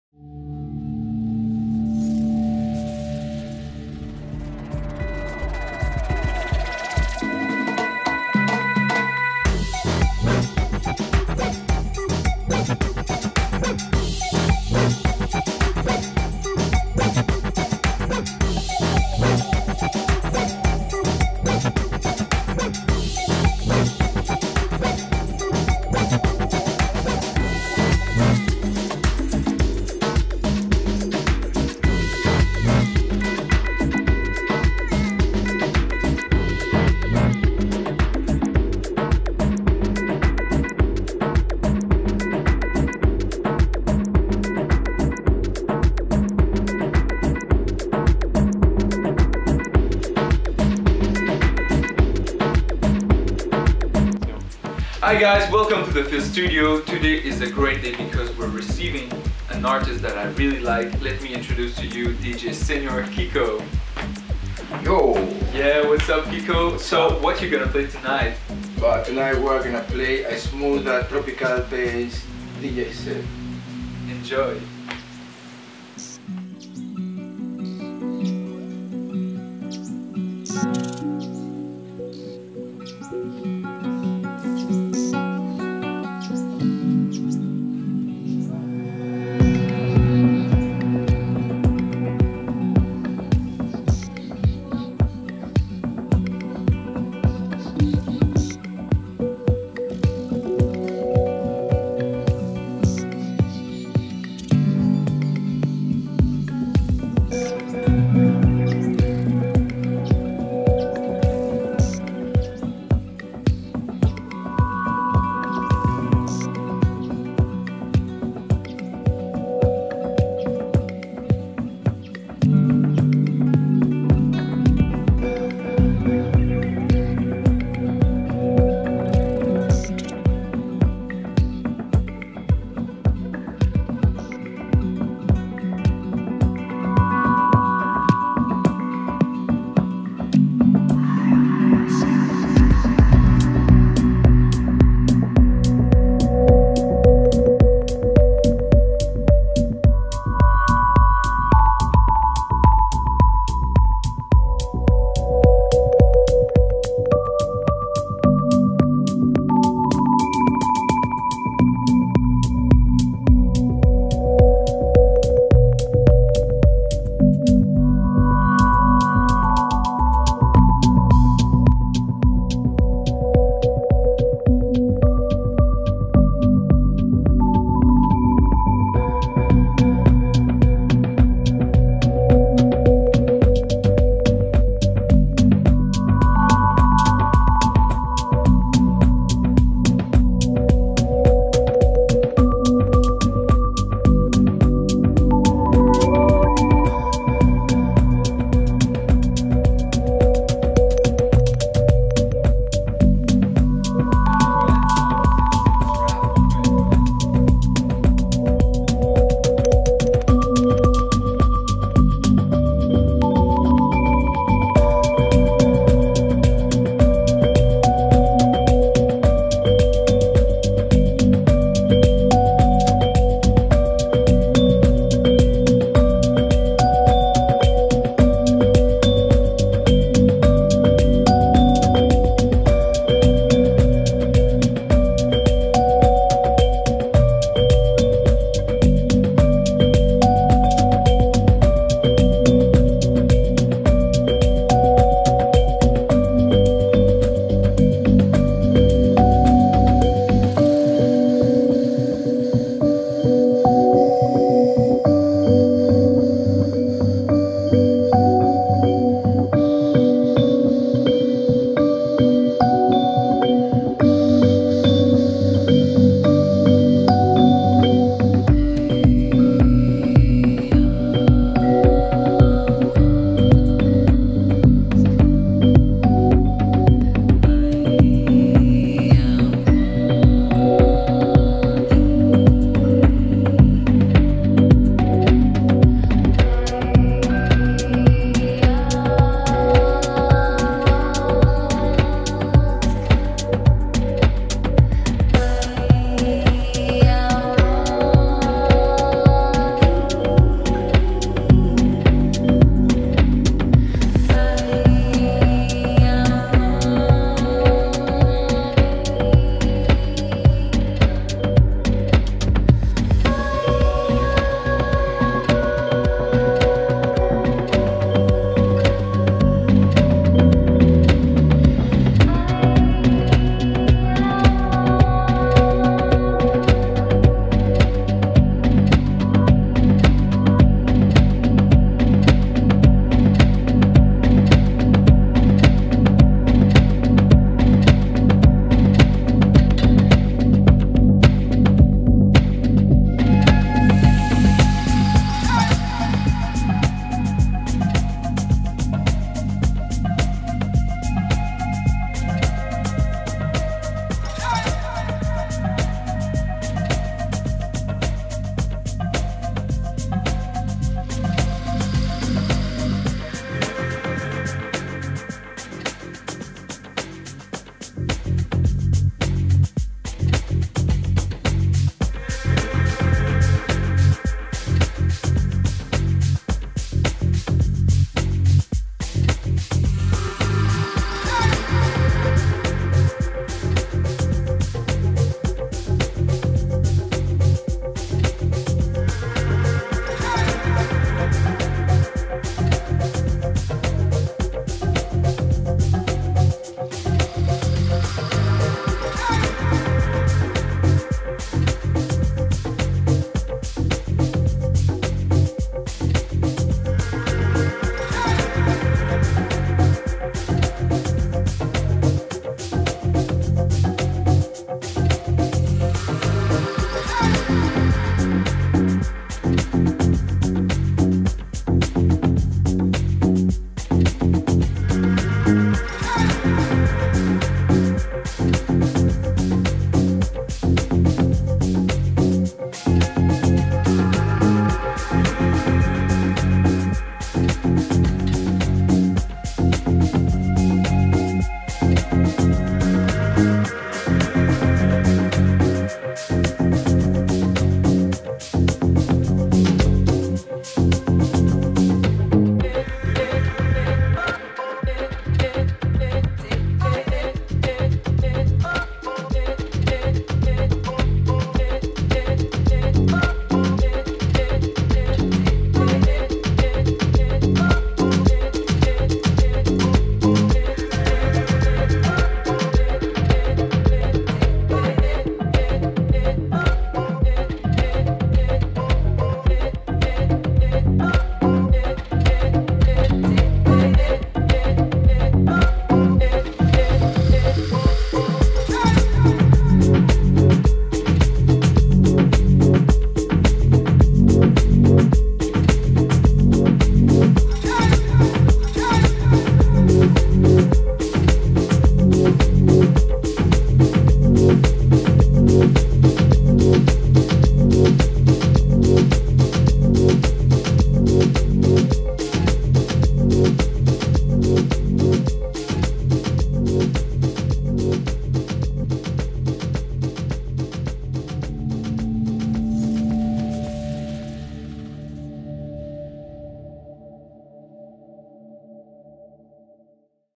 DJ Señor KIKO in the Phil Studio
ThePhilStudio has received a special artist and we are presenting to you this unique performance.

DJ Señor KIKO a Mexican DJ based in Montreal, Presenting the new electronic latin sound with rhythms like Tropical Bass, Electro Cumbia and Moombahton.

We welcomed him in our studio so he could perform a smooth tropical bass DJ set.

Special thanks to Mat Thel for the collaboration with cameras and visual effects.